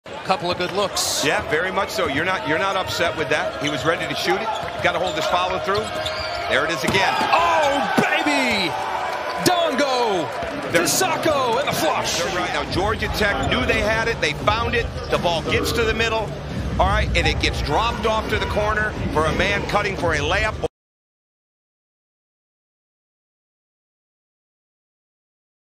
Georgia Tech's Ibrahima Sacko Dunks Down The Hockey Assist
Check out Georgia Tech executing to perfection as Naithan George throws a sweet dish to Baye Ndongo, who finds Ibrahima Sacko who flushes it down with authority.